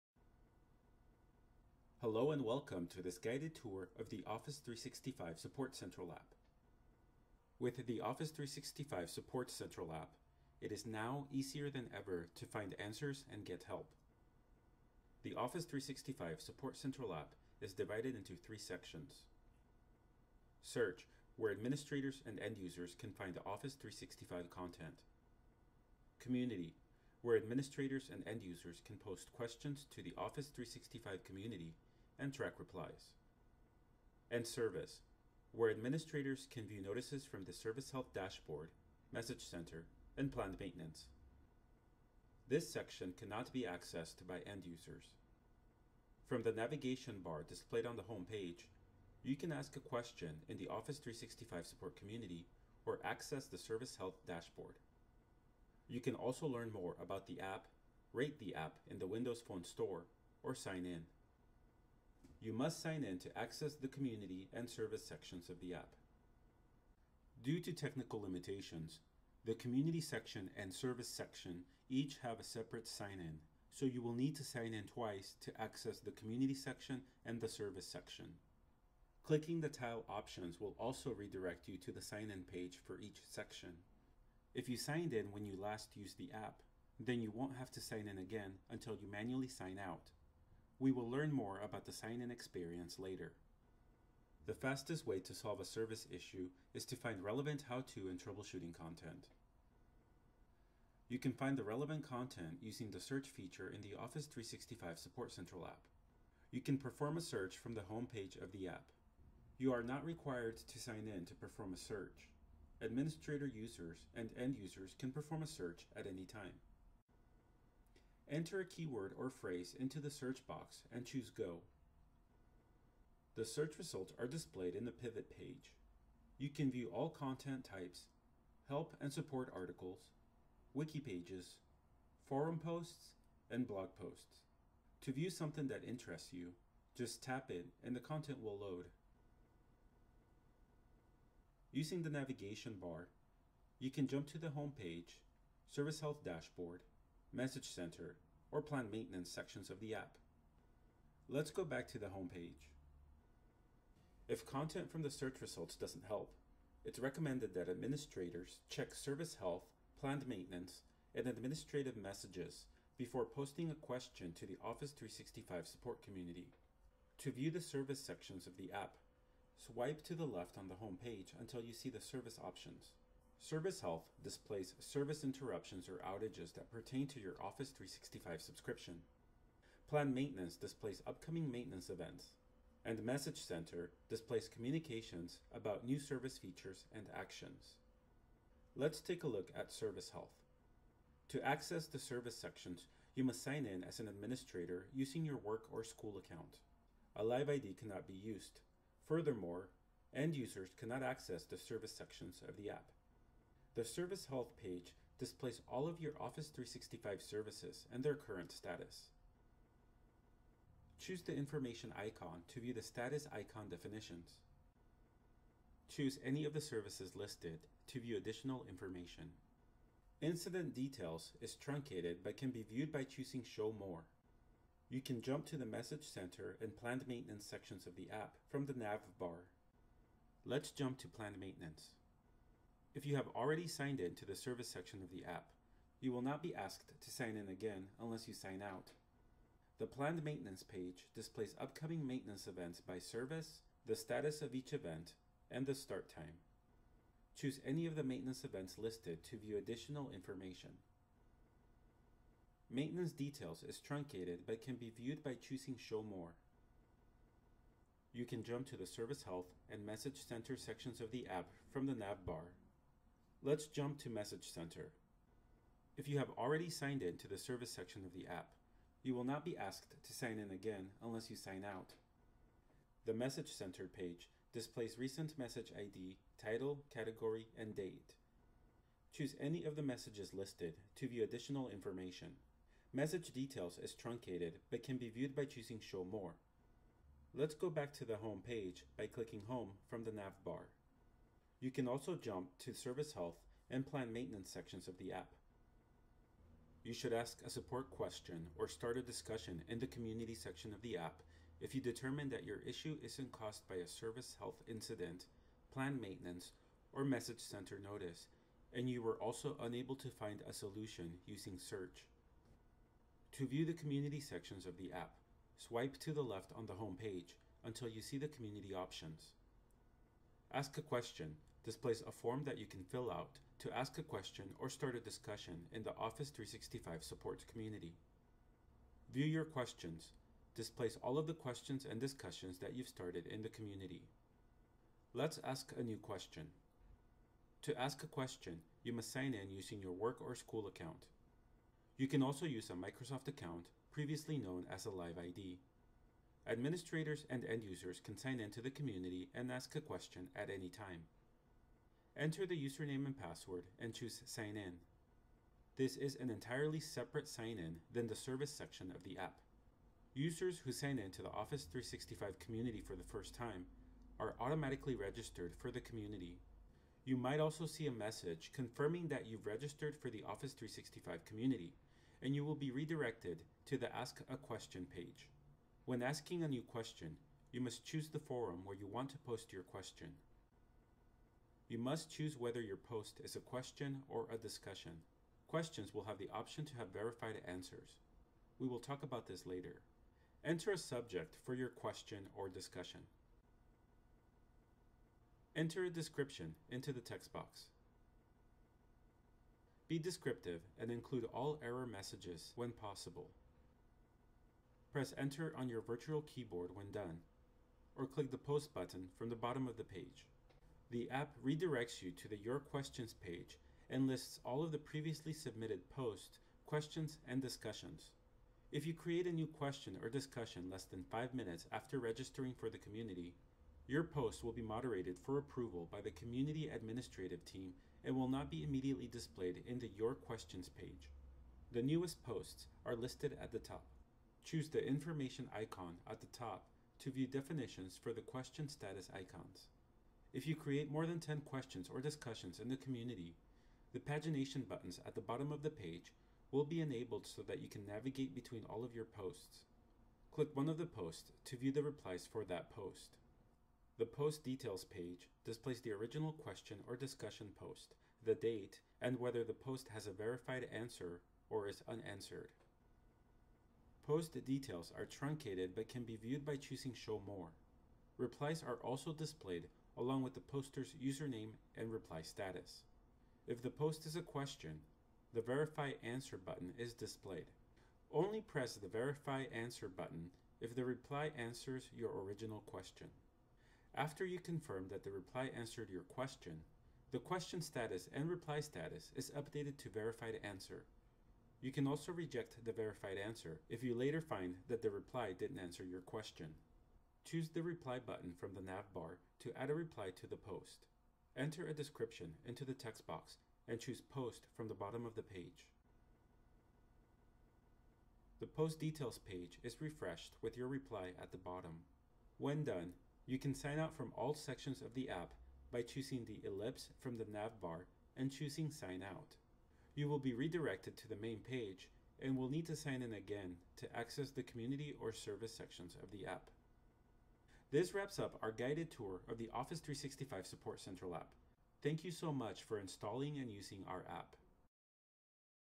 Office 365 Support Central App Guided Tour
The Office 365 Support Central mobile app allows you to:

• Search for answers in the community, help and troubleshooting articles.

• View service health incidents, planned maintenance events, and message center notices.

• Easily track only your own questions and answers from the community.